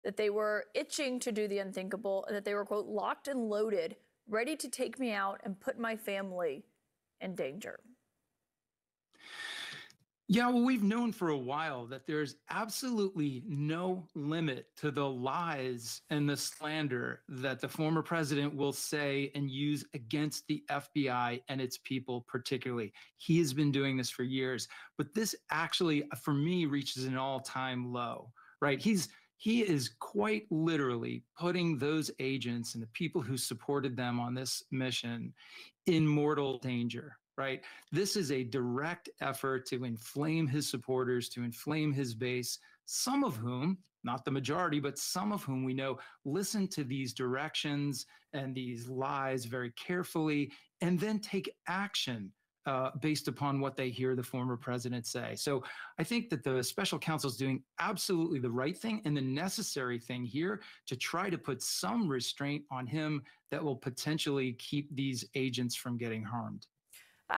Former President's Dangerous Lies: Exposing the FBI and Putting Agents at Risk
Discover the shocking truth behind the former president's relentless attacks on the FBI. Learn how his false statements not only endanger the lives of agents but also incite his base to take dangerous actions. Find out why the special counsel's intervention is crucial in preventing harm.